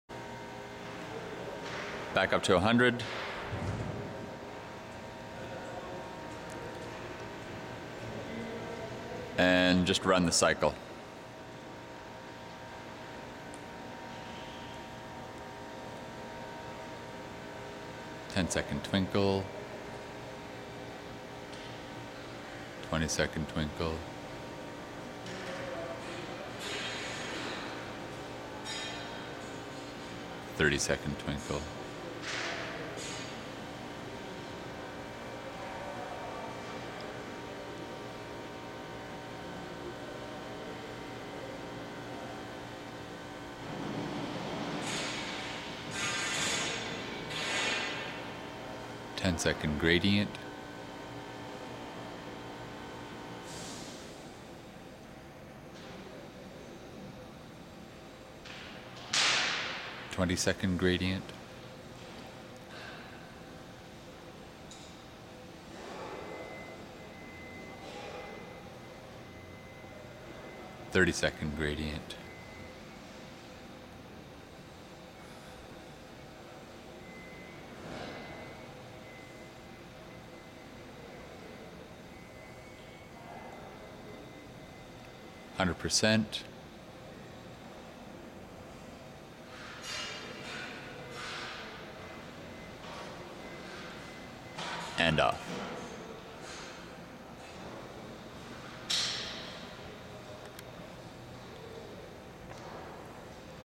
Chinook Centre chandelier - close-up of icicles
Shows the twinkle and gradient effects close-up.  When chandelier is off at end of video the LEDs boards are visible, they disappear as the video is running.